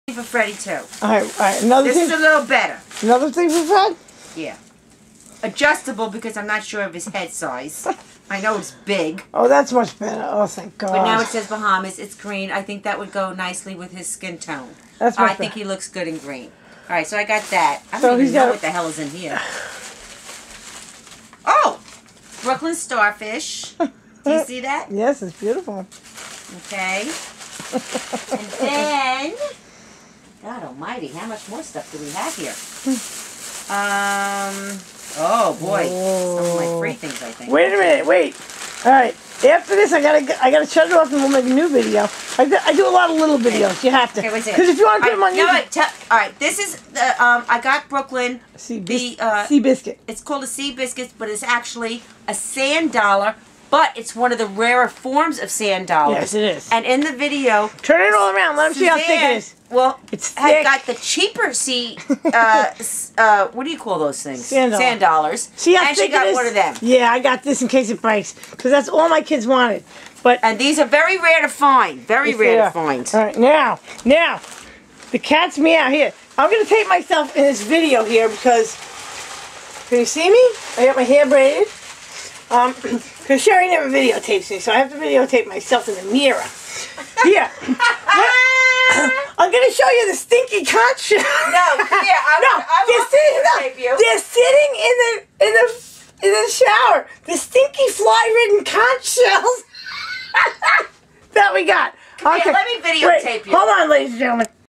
Sue and Sherry Cruise August 2010 - Shopping purchases leading to the CONCH SHELL
Continuing to look over all the deals we got at Nassau, Bahamas.